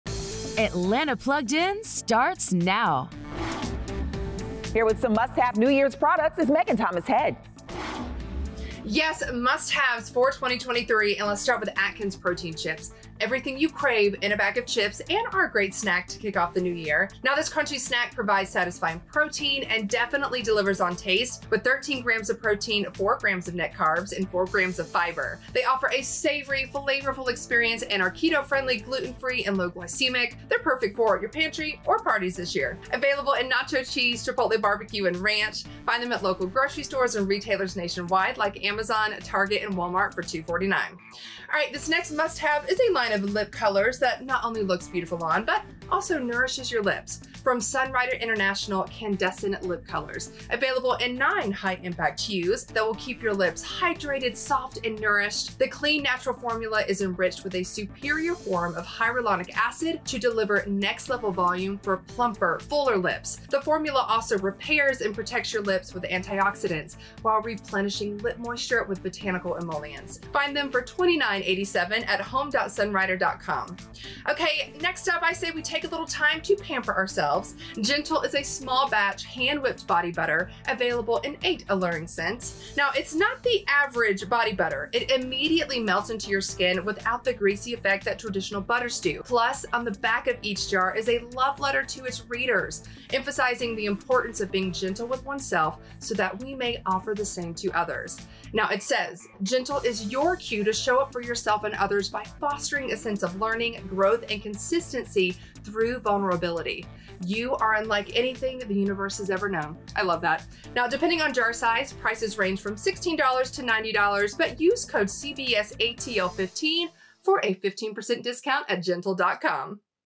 Must-Have Products for 2023 with Megan Thomas Head
Lifestyle Expert, Megan Thomas Head is here to share a few must-have products that are perfect for the new year.